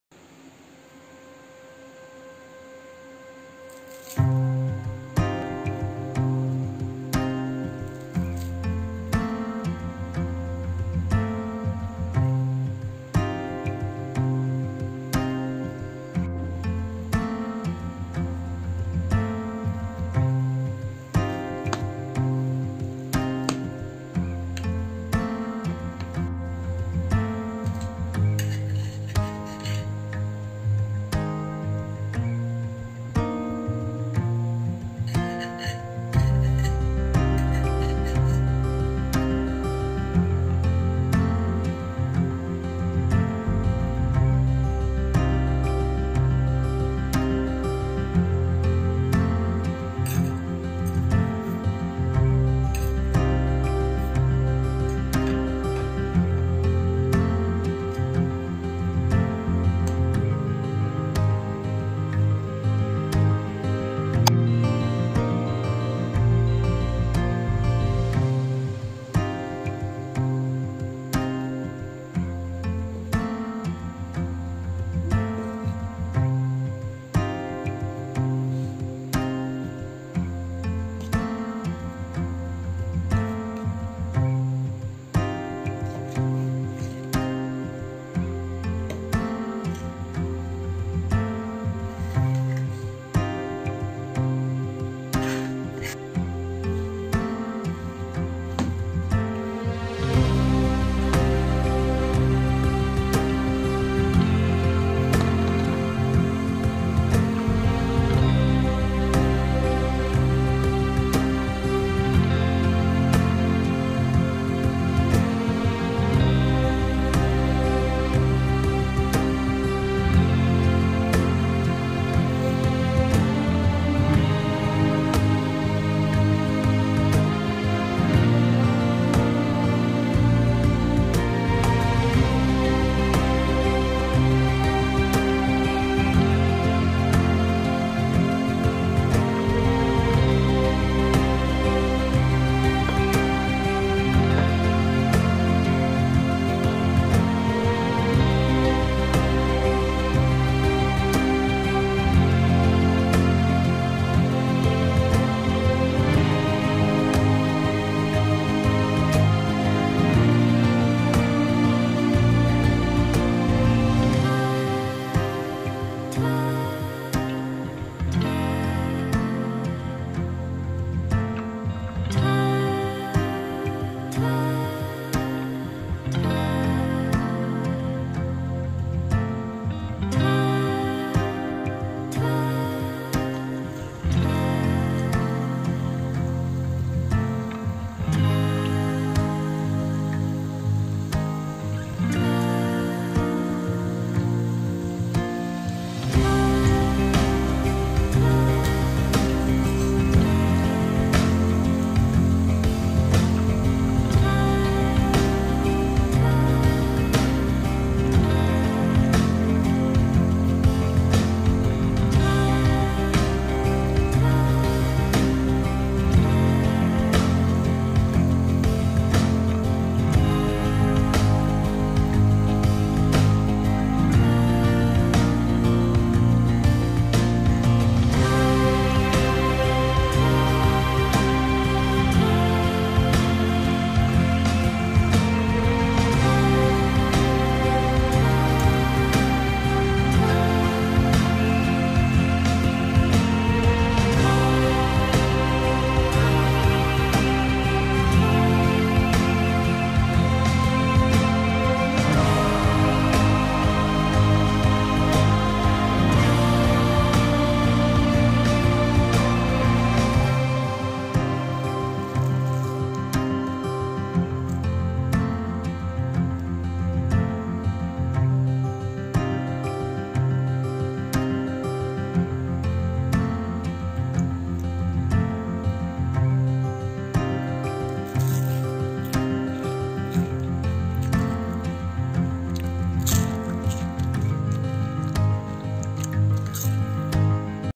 Garlic potato ring | Fried potatoes | Crispy potatoes snacks
I made soft but crunchy potato rings.
The scent of garlic butter and coriender leaves goes well together, and the flavor is very good.
It's easy to make, so make it!
It's great as a snack or side dish

Ingredient

Potatoes
garlic
Unsalted butter
Salt
rice flour
corn flour

GarlicPotato Ring
Fried Potatoes